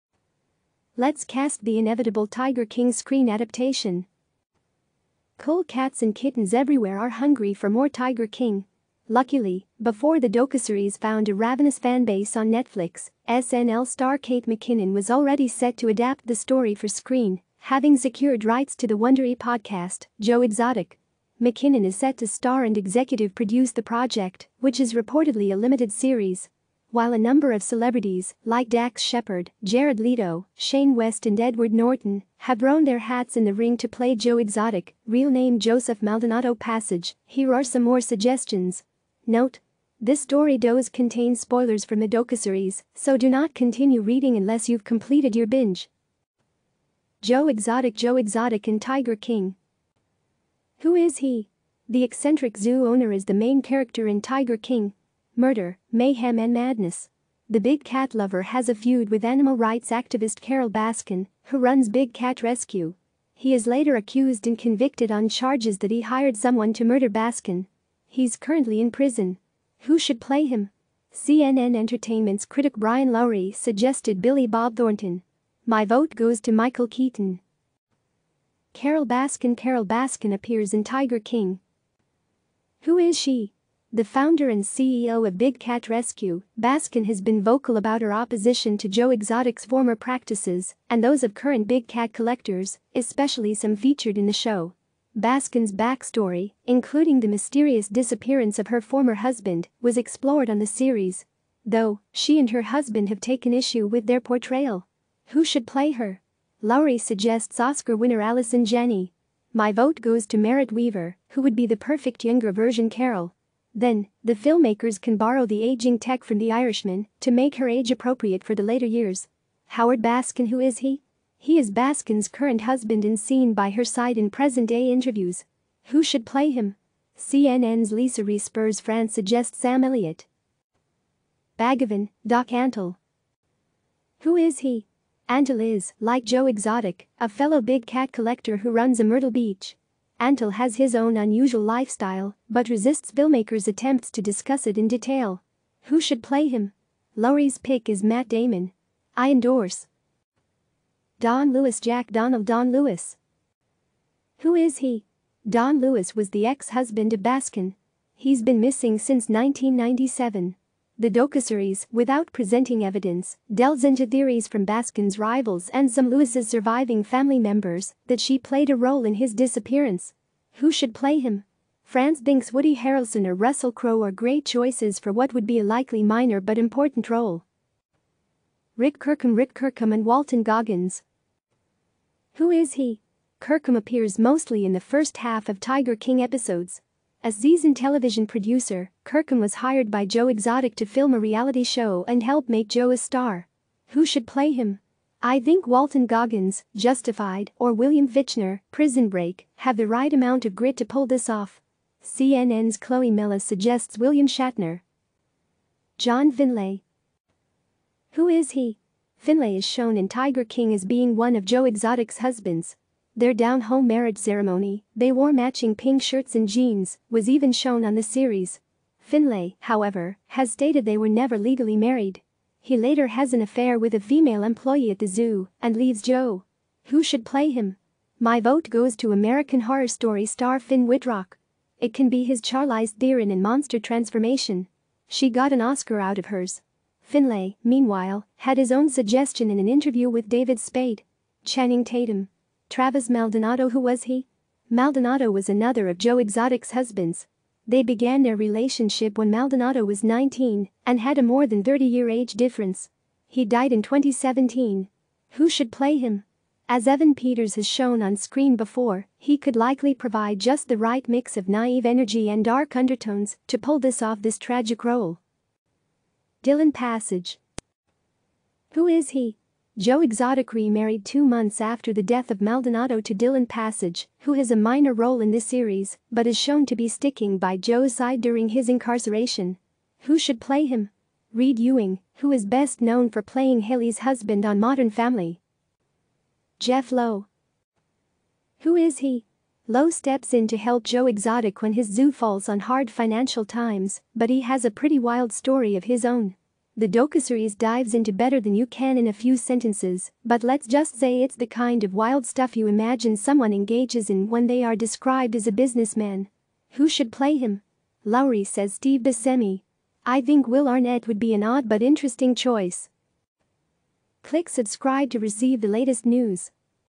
Let's cast the inevitable 'Tiger King' screen adaptation
Cool cats and kittens everywhere are hungry for more "Tiger King."

Luckily, before the docuseries found a ravenous fanbase on Netflix, "SNL" star Kate McKinnon was already set to adapt the story for screen, having secured rights to the Wondery podcast "Joe Exotic."

McKinnon is set to star and executive produce the project, which is reportedly a limited series.

While a number of celebrities -- like Dax Shepard, Jared Leto, Shane West and Edward Norton -- have thrown their hats in the ring to play Joe Exotic (real name Joseph Maldonado-Passage), here are some more suggestions. (Note: This story does contain spoilers from the docuseries, so do not continue reading unless you've completed your binge.)

Joe Exotic

Joe Exotic in 'Tiger King'

Who is he? The eccentric zoo owner is the main character in "Tiger King: Murder, Mayhem and Madness." The big cat lover has a feud with animal rights activist Carole Baskin, who runs Big Cat Rescue. He is later accused and convicted on charges that he hired someone to murder Baskin. He's currently in prison.

Who should play him? CNN Entertainment's critic Brian Lowry suggested Billy Bob Thornton. My vote goes to Michael Keaton.

Carole Baskin

Carole Baskin appears in "Tiger King."

Who is she? The founder and CEO of Big Cat Rescue, Baskin has been vocal about her opposition to Joe Exotic's former practices and those of current big cat collectors, especially some featured in the show. Baskin's backstory -- including the mysterious disappearance of her former husband -- was explored on the series. Though, she and her husband have taken issue with their portrayal.

Who should play her? Lowry suggests Oscar-winner Allison Janney. My vote goes to Merritt Wever, who would be the perfect younger version Carole. Then, the filmmakers can borrow the aging tech from "The Irishman" to make her age appropriate for the later years.

Howard Baskin

Who is he? He is Baskin's current husband and seen by her side in present-day interviews.

Who should play him? CNN's Lisa Respers France suggests Sam Elliott.

Bhagavan "Doc" Antle

Who is he? Antle is, like Joe Exotic, a fellow big cat collector who runs a Myrtle Beach. Antle has his own unusual lifestyle but resists filmmaker's attempts to discuss it in detail.

Who should play him? Lowry's pick is Matt Damon. I endorse.

Don Lewis

Jack Donald "Don" Lewis

Who is he? Don Lewis was the ex-husband of Baskin. He's been missing since 1997. The docuseries, without presenting evidence, delves into theories from Baskin's rivals and some Lewis' surviving family members that she played a role in his disappearance.

Who should play him? France thinks Woody Harrelson or Russell Crowe are great choices for what would be a likely minor but important role.

Rick Kirkham

Rick Kirkham and Walton Goggins

Who is he? Kirkham appears mostly in the first half of "Tiger King" episodes. A seasoned television producer, Kirkham was hired by Joe Exotic to film a reality show and help make Joe a star.

Who should play him? I think Walton Goggins ("Justified") or William Fitchner ("Prison Break") have the right amount of grit to pull this off. CNN's Chloe Melas suggests William Shatner.

John Finlay

Who is he? Finlay is shown in "Tiger King" as being one of Joe Exotic's husbands. Their down-home marriage ceremony (they wore matching pink shirts and jeans) was even shown on the series. (Finlay, however, has stated they were never legally married.) He later has an affair with a female employee at the zoo and leaves Joe.

Who should play him? My vote goes to "American Horror Story" star Finn Witrock. It can be his Charlize Theron in "Monster" transformation. She got an Oscar out of hers. Finlay, meanwhile, had his own suggestion in an interview with David Spade: Channing Tatum.

Travis Maldonado

Who was he? Maldonado was another of Joe Exotic's husbands. They began their relationship when Maldonado was 19 and had a more than 30 year age difference. He died in 2017.

Who should play him? As Evan Peters has shown on screen before, he could likely provide just the right mix of naive energy and dark undertones to pull this off this tragic role.

Dillon Passage

Who is he? Joe Exotic re-married two months after the death of Maldonado to Dillon Passage, who has a minor role in this series but is shown to be sticking by Joe's side during his incarceration.

Who should play him? Reid Ewing, who is best known for playing Haley's husband on "Modern Family."

Jeff Lowe

Who is he? Lowe steps in to help Joe Exotic when his zoo falls on hard financial times, but he has a pretty wild story of his own. The docuseries dives into better than you can in a few sentences, but let's just say it's the kind of wild stuff you imagine someone engages in when they are described as "a business man."

Who should play him? Lowry says Steve Buscemi. I thin